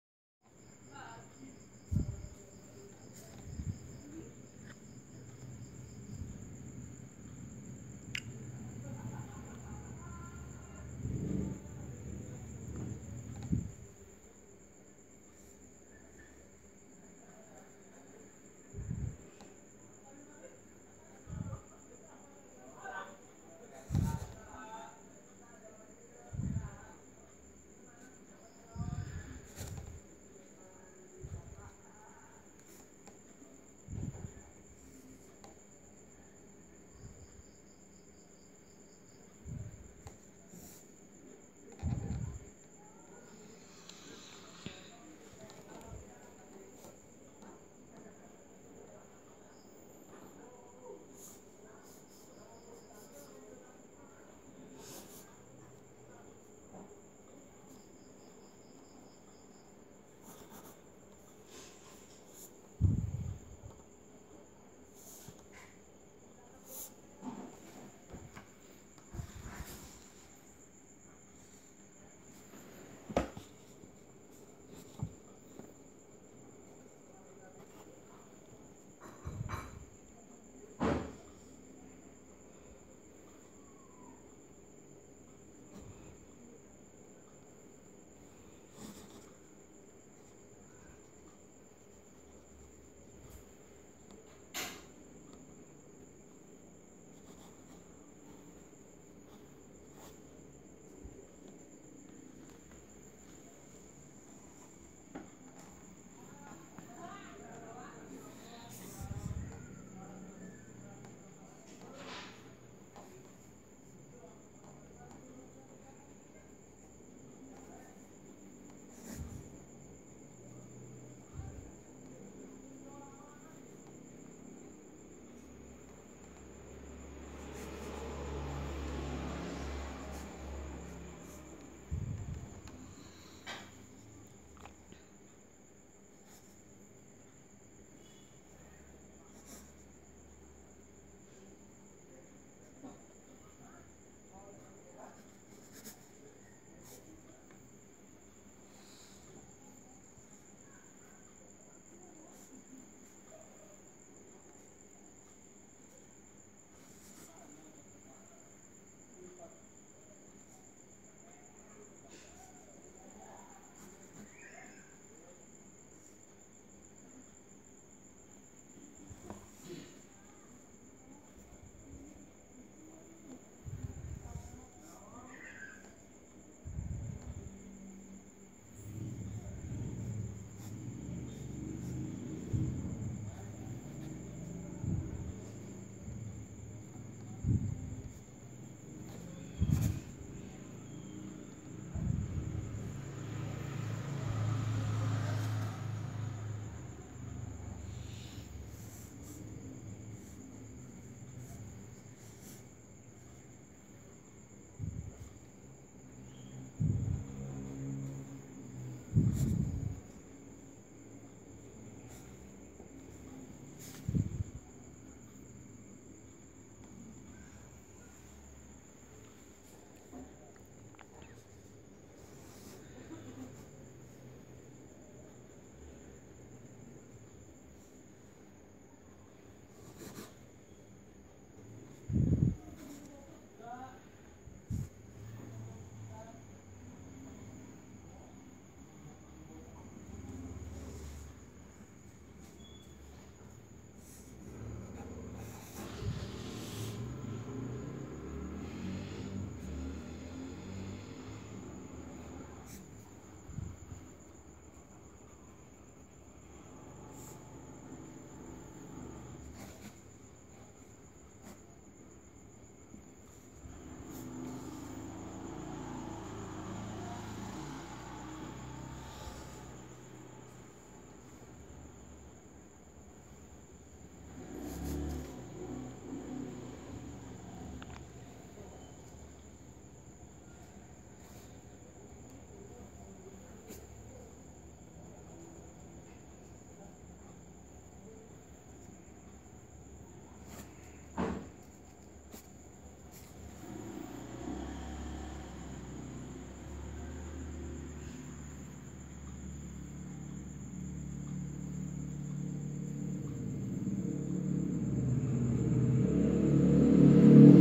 Signature Request Nuraeni |Tanda Tangan Request Nuraeni
Terimakasih banyak Nuraeni nih saya kasih banyak inspirasi Tanda Tangan. yu silahkan komentar bagi kalian yang ingin Inspirasi dariku. Terimakasih